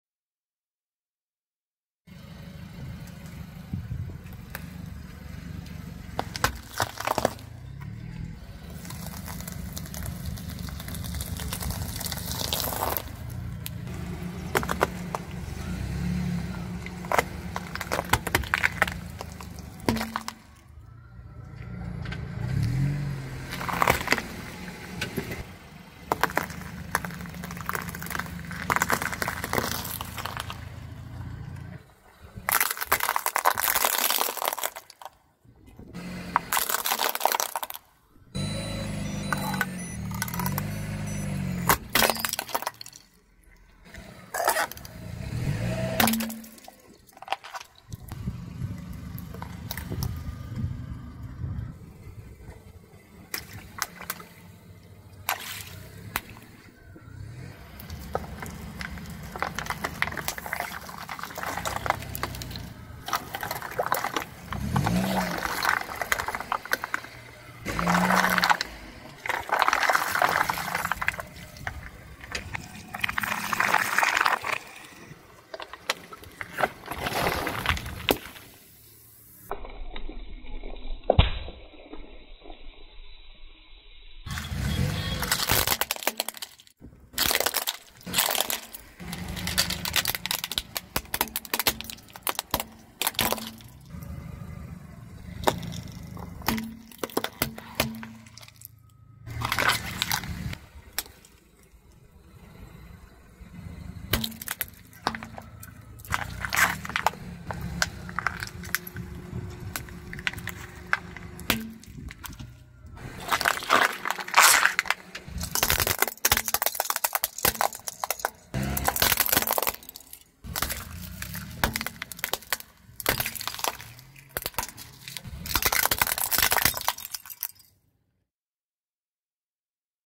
Aplastando Cosas Crujientes y Suaves con la Rueda de un Coche
En este EXPERIMENTO veremos Aplastando Cosas Crujientes y Suaves con la Rueda de un Coche, yo soy Alvaro y esto es Test ALVARO